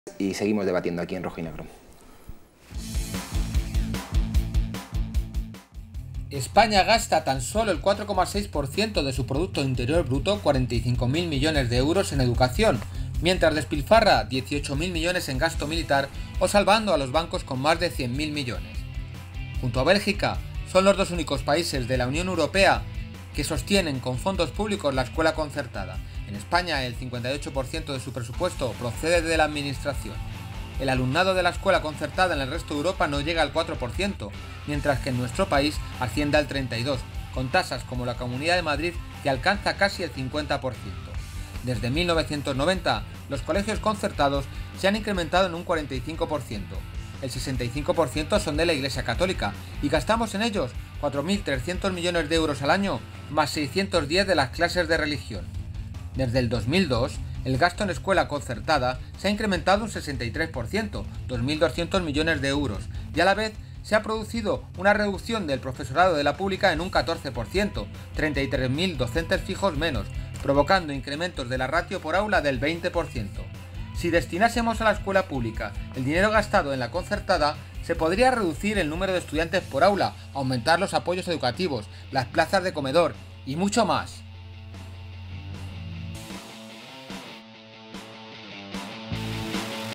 RNtv Educación Concertada vs Pública
España gasta tan solo el 4,6% de su Producto Interior Bruto (45.000 millones de €) en educación, mientras despilfarra 18.000 millones en gasto militar o 100.000 millones salvando Bancos.
Junto a Bélgica, son los dos únicos países de la Unión Europea que sostienen con fondos públicos la escuela concertada. En España el 58% de su presupuesto procede de la administración.

El alumnado de la escuela concertada en el resto de Europa no llega al 4%, mientras que en nuestro país asciende al 32%, con tasas, como la Comunidad de Madrid, que llegan al 50%.
Desde 1990 hay un 45% más de colegios concertados, el 65% de la Iglesia Católica, en los que gastamos 4.300 millones de € al año más 610 de las clases de religión.

Desde el 2002, el gasto de la concertada se incrementa un 63% y el de la pública se reduce el 14%: 33.000 docentes fijos menos, incrementos de la ratio por aula del 20%.
Todo este dinero destinado a la pública podría reducir el número de estudiantes por aula, aumentar los apoyos educativos, las plazas de comedor y mucho más.